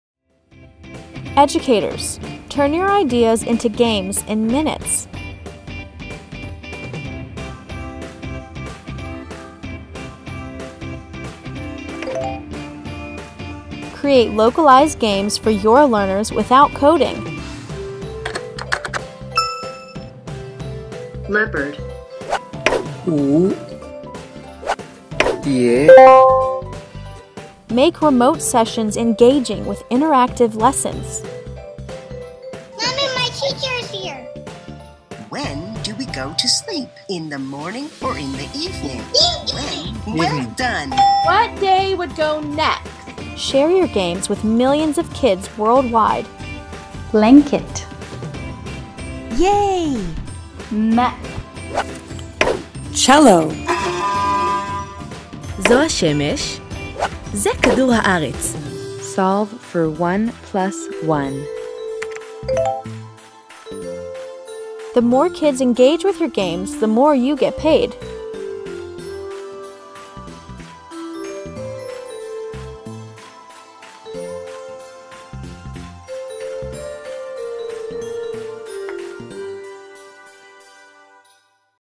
Teachers! create TinyTap games for your learners without coding
Download TinyTap on the iPad and start creating in minutes with no need of coding